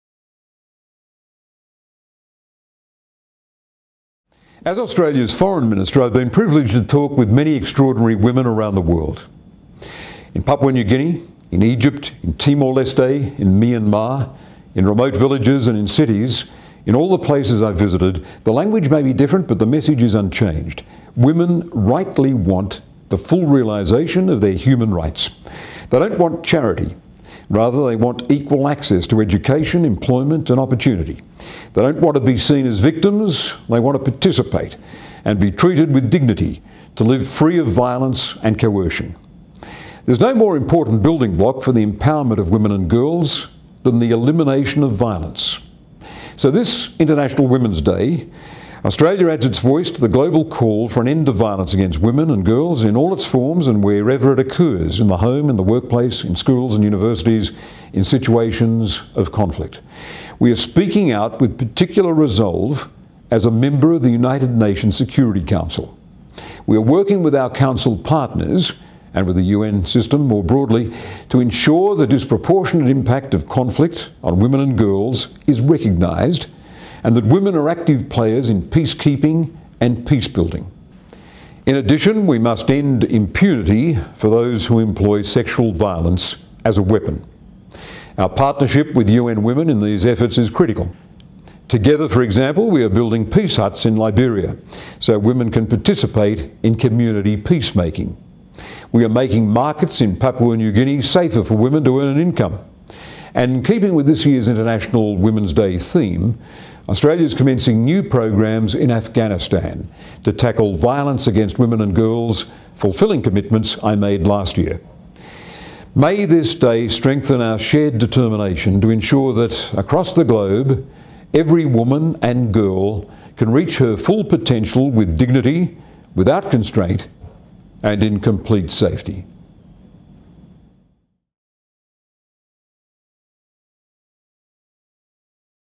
UN International Women's Day Message 2013
UN International Women's Day 2013: Message by Senator the Hon Bob Carr Australian Minister for Foreign Affairs.

8 March 2013